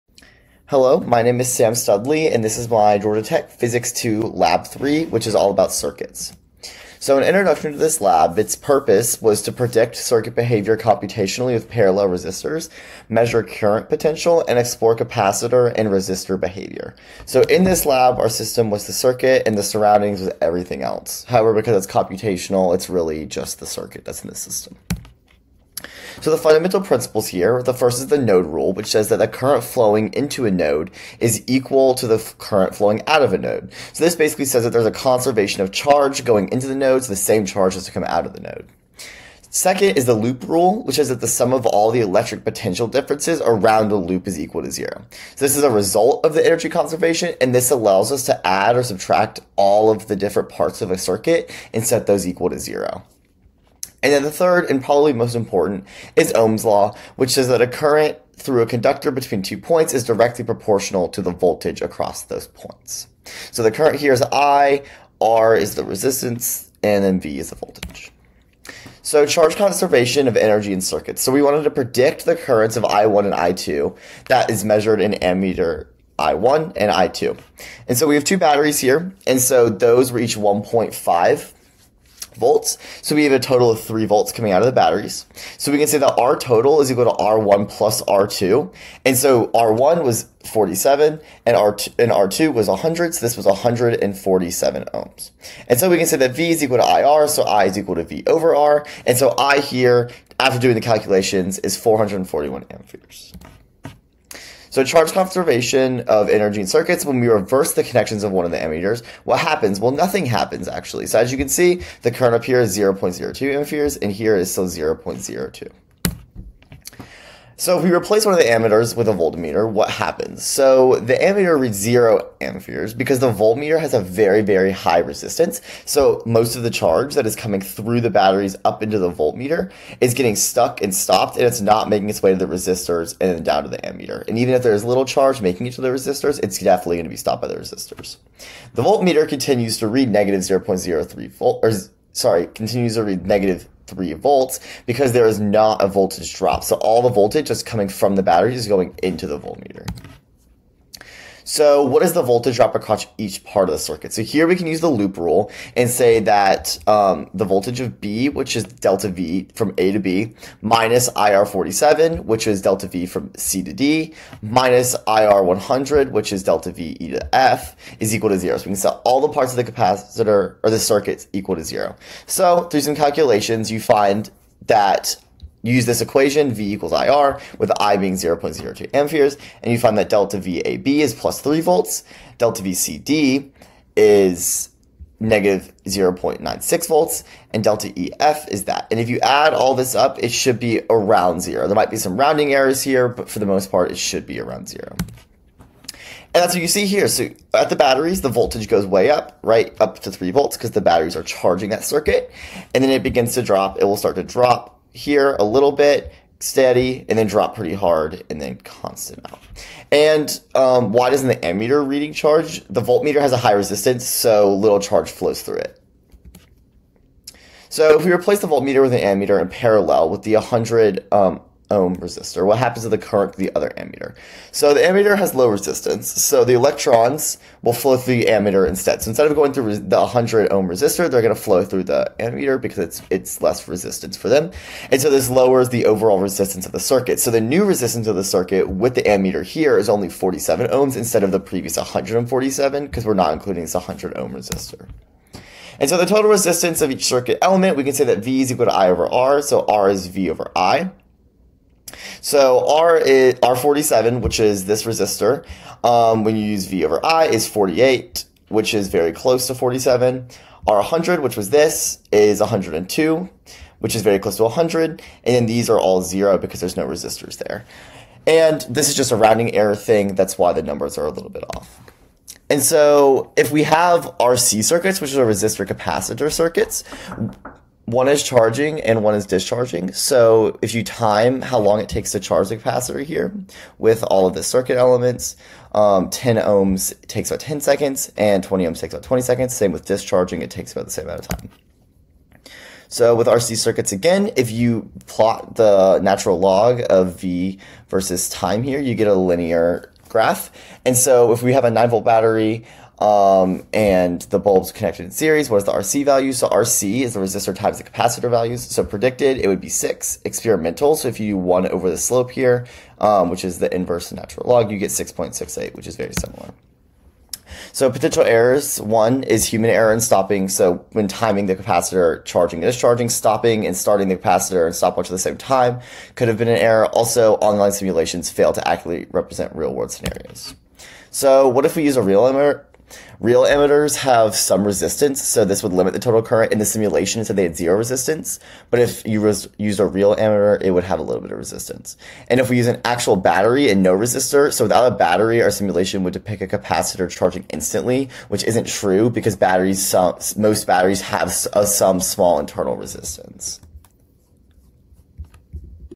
Georgia Tech PHYS 2212 Lab 3: Circuits
This lab explores circuits and models them computationally.